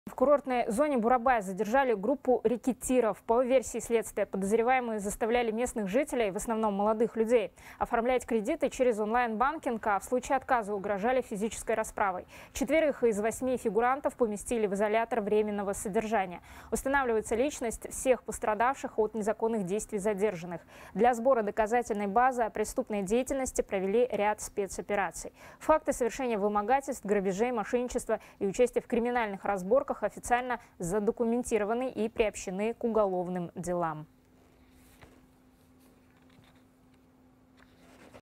Группу рэкетиров задержали в Бурабае
В курортной зоне «Бурабай» задержали группу рэкетиров, сообщает «Хабар 24». По версии следствия, подозреваемые заставляли местных жителей, в основном молодых людей, оформлять кредиты через онлайн-банкинг, а в случае отказа угрожали физической расправой.